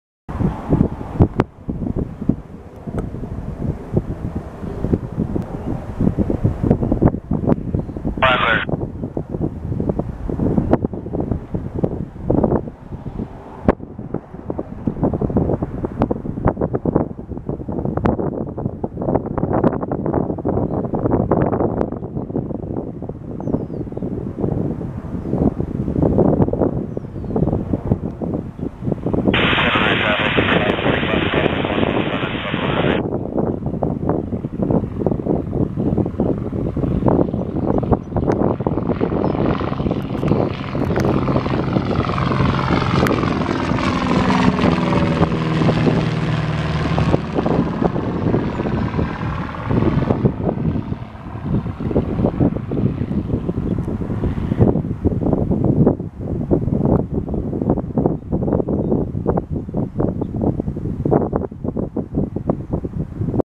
Cessna 172G Skyhawk Take-Off at Central Jersey Regional Airport (47N)
Here is a 1966 Cessna 172G Skyhawk taking off on Runway 7 at Central Jersey Regional Airport (47N) in Hillsborough, NJ for a flight in the traffic pattern this evening shortly upon my arrival on the field.

Aircraft: Cessna 172G Skyhawk

Registration: N4222L